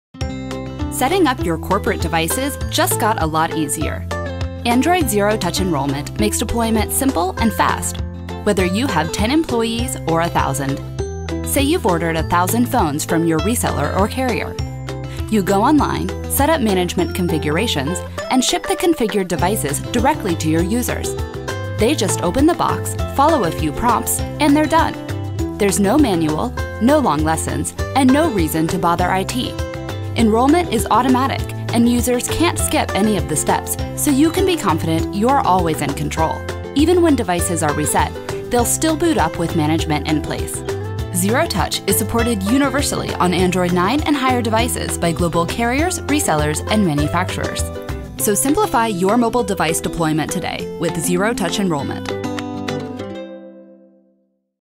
Skywire Australia - Zero-touch Enrolment Overview
Skywire Australia

This video from Google explains the basic benefits of Android Zero-touch.
Skywire goes a step beyond by ensuring devices are ready to go as soon as they are unboxed - no additional prompts.